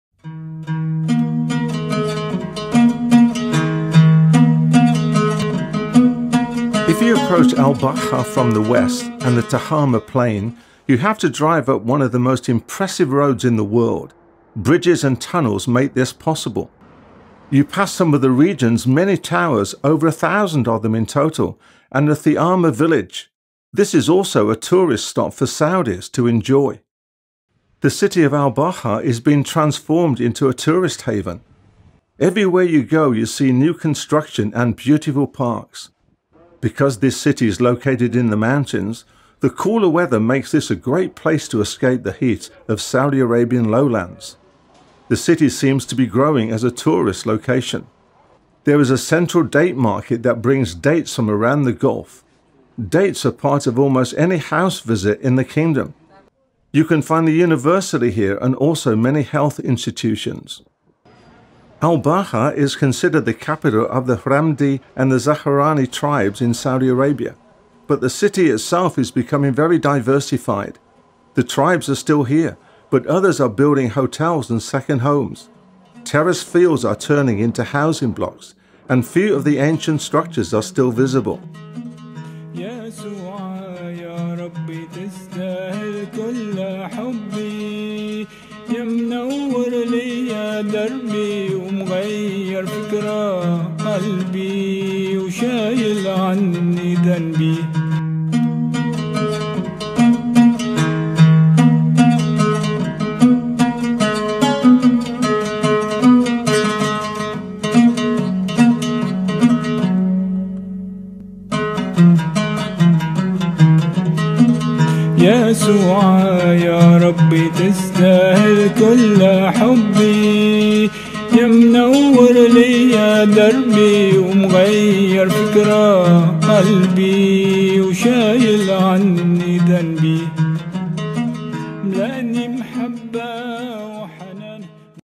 Al Baha, Saudi Arabia
Calling Christians to pray for Al Baha, Saudi Arabia

إن كنت ترغب في معرفة المزيد عن المسيح كما يقدمه الكتاب المقدس يُرجى زيارة هذا الموقع